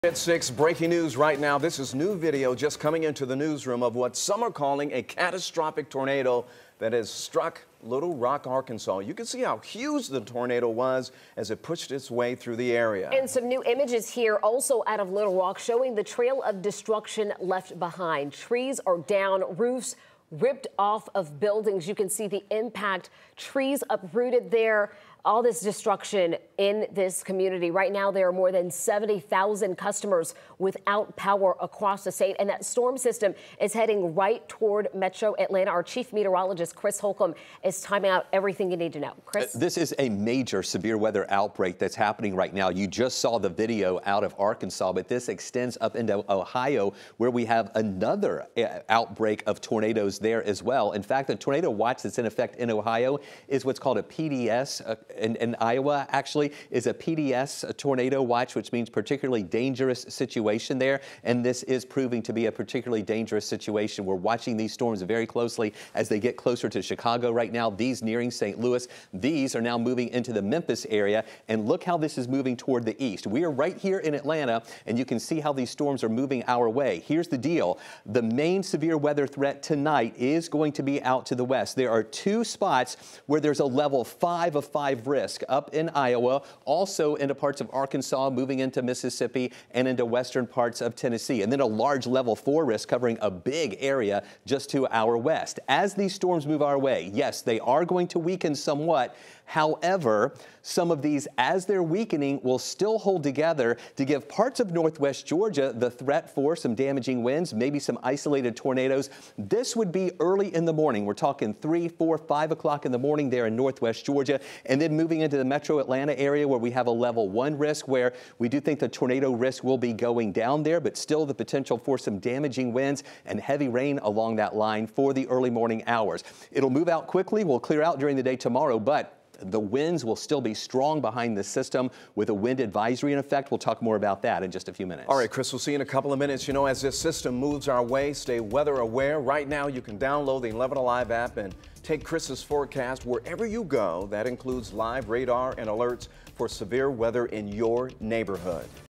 Severe weather in Arkansas, storm system heads toward Georgia
Northwest Georgia could see the threat of tornadoes overnight.